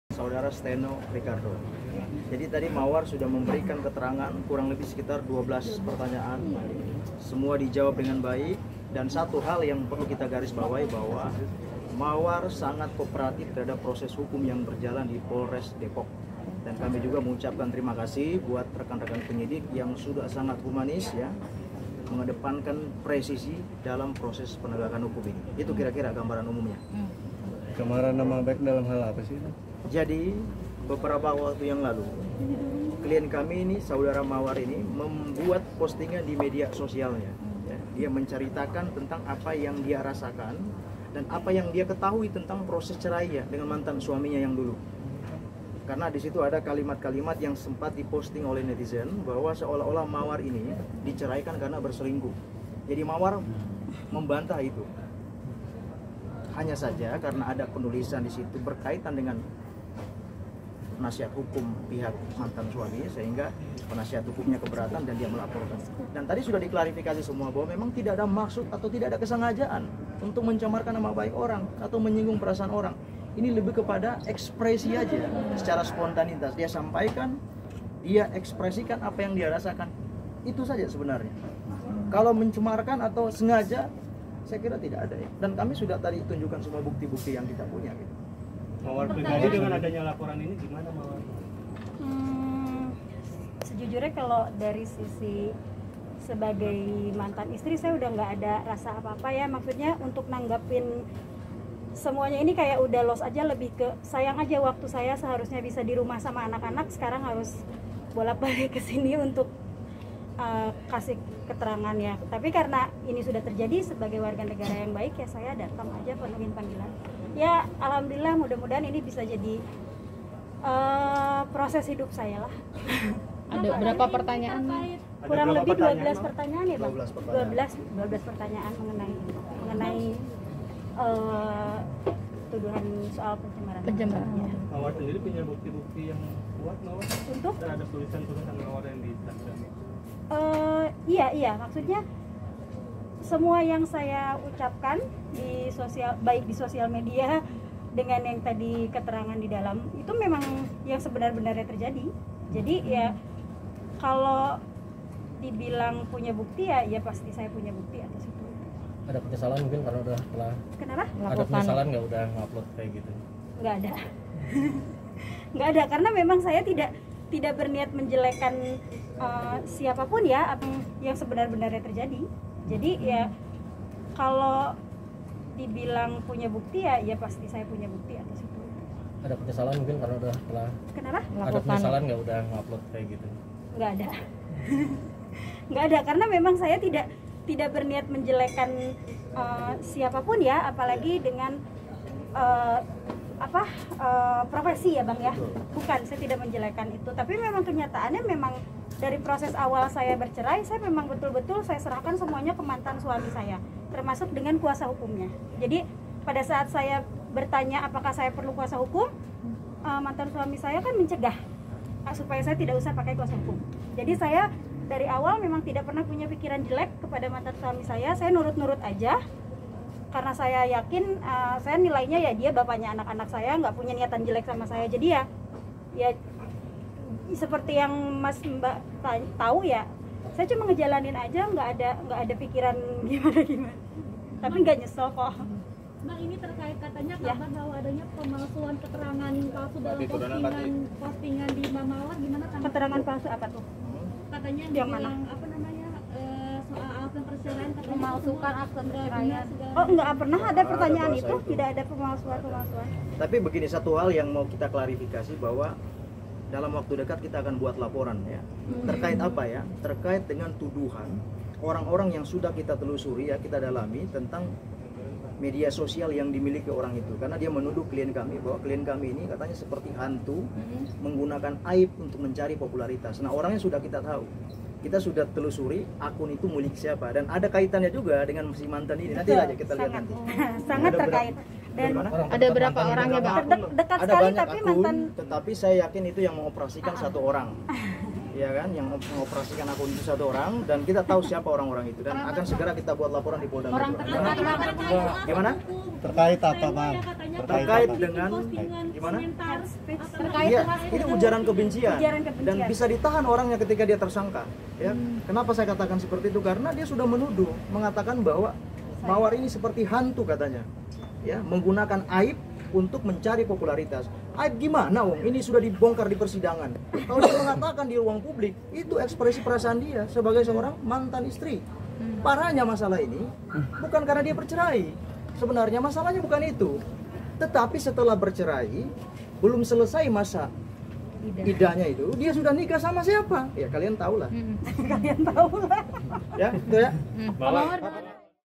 Mawar AFI Sanggah Dituding Cemarkan Nama Baik
Penyanyi Mawar AFI selesai diperiksa penyidik Polres Metro Depok terkait kasus dugaan pencemaran nama baik pada Selasa (5/4/2022).

Dalam pemeriksaan hari ini, Penyanyi jebolan ajang pencarian bakat itu telah memberikan keterangannya di dalam ruang pemeriksaan.

Zakir Rasyidin selaku kuasa hukum dari Mawar AFI mengungkapkan bahwa kliennya hari ini mendapatkan 12 pertanyaan saat pemeriksaan.